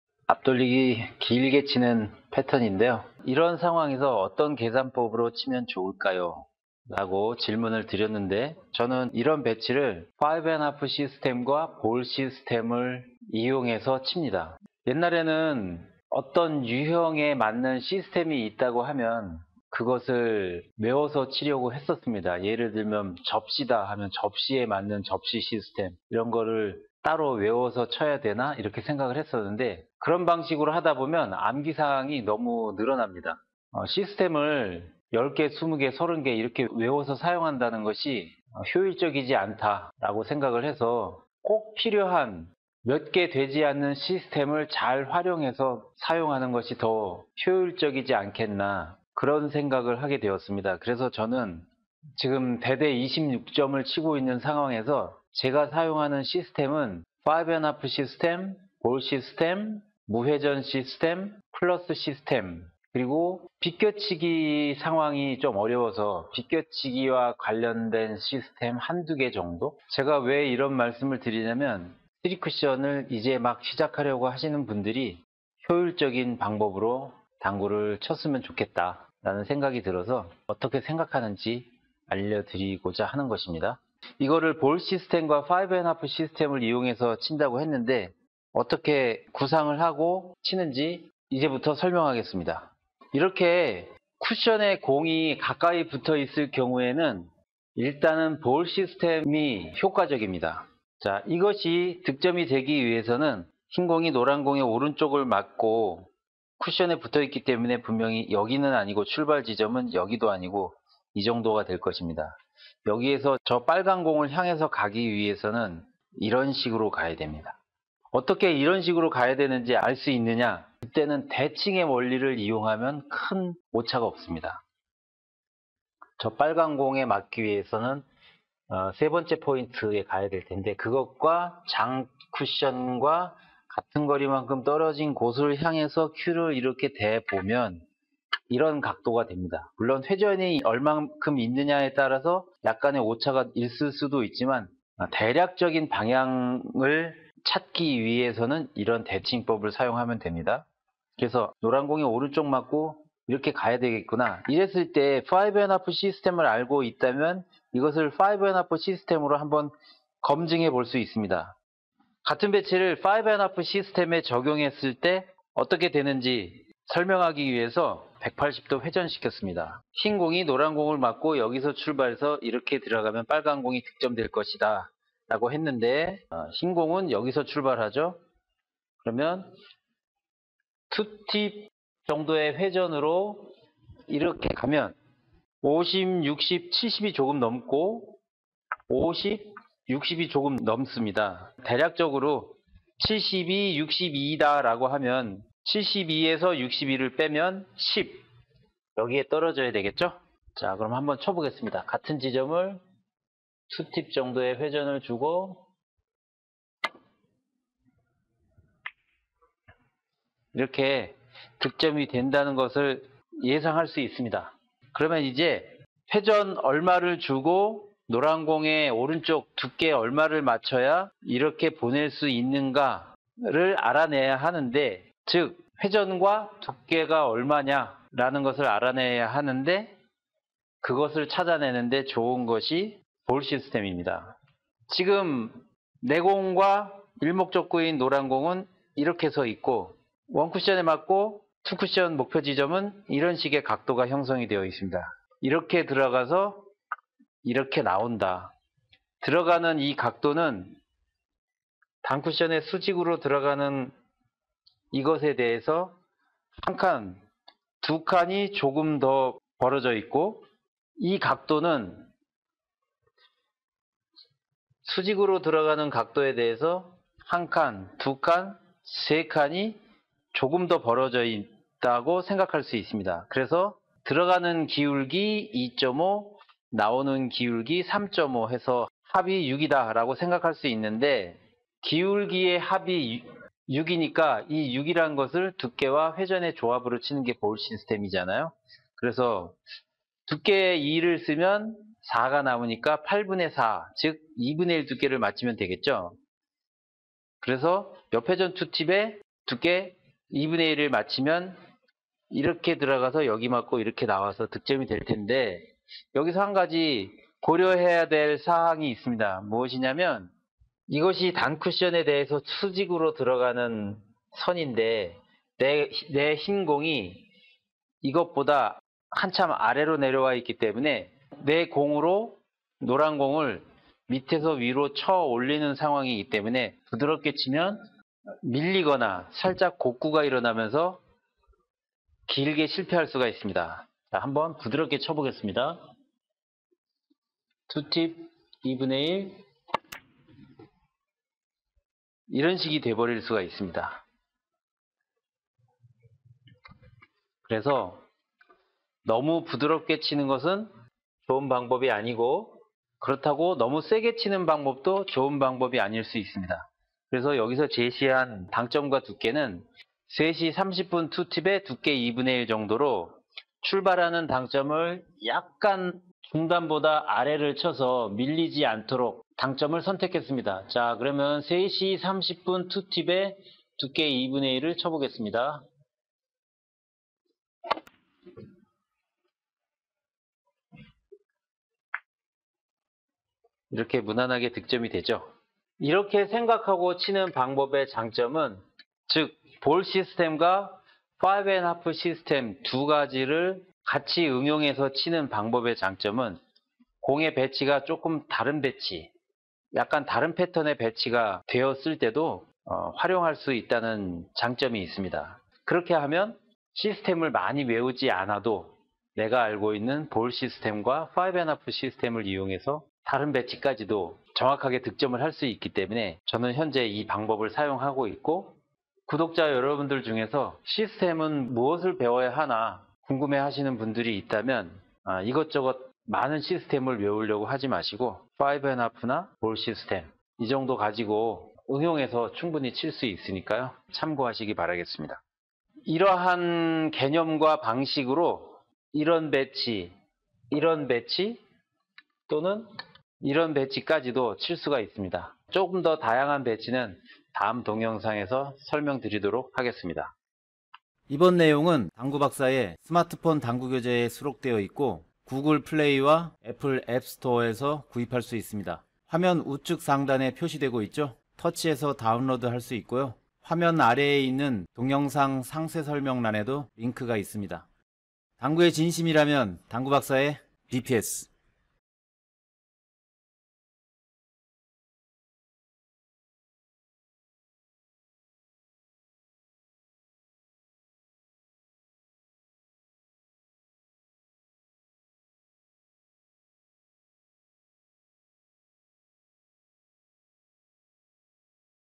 앞돌리기 3-1 - 길게 치는 앞돌리기 5&하프시스템과 볼시스템을 응용한 공략법
01:25 쓰리쿠션 앞돌리기 길게 공략법 구상하는 방법
06:25 이 볼시스템과 5&하프시스템 활용법의 장점
스마트폰 당구교재(당구박사) 중 앞돌리기 3-1로 수록된 배치입니다. 앞돌리기 길게 치는 상황에서 볼시스템과 5&하프시스템을 이용하여 계산하고 득점하는 요령을 알려드립니다. 시스템을 처음으로 배우려고 하는 분들 참고하세요. URL

스마트폰당구교재 당구박사 (Dr.Billiard's lesson master)